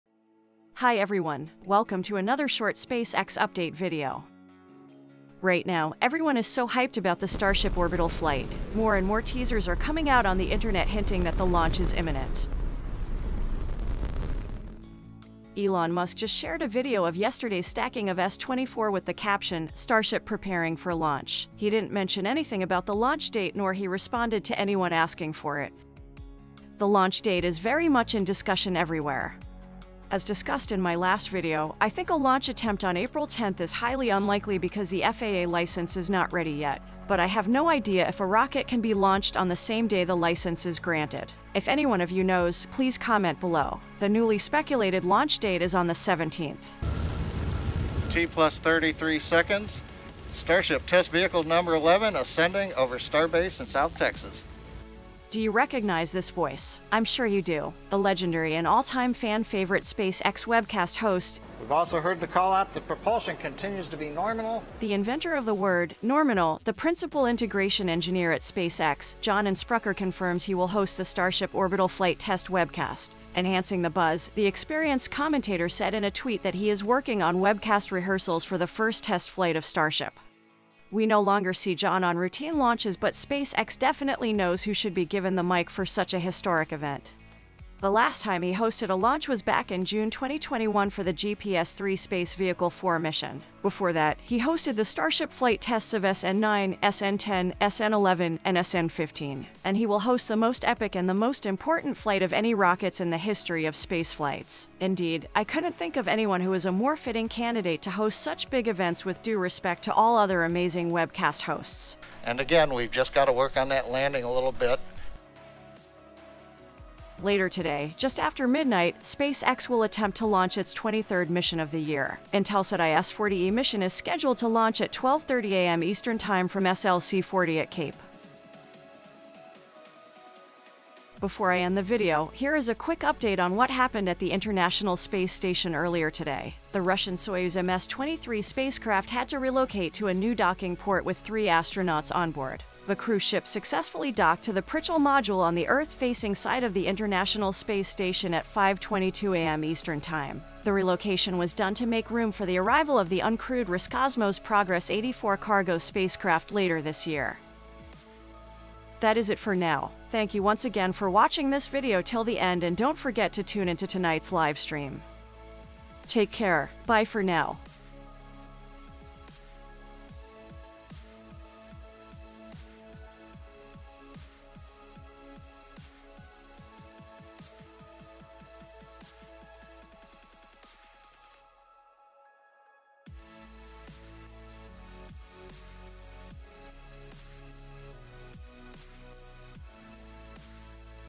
Elon Musk : Starship Preparing For Launch 🚀 | Starship OTF Official Webcast Host Revealed | SpaceX
Elon Musk says SpaceX is preparing for the Starship Launch. The official webcast host of the maiden historic flight has also been revealed.
Watch the video to find out who is he/she.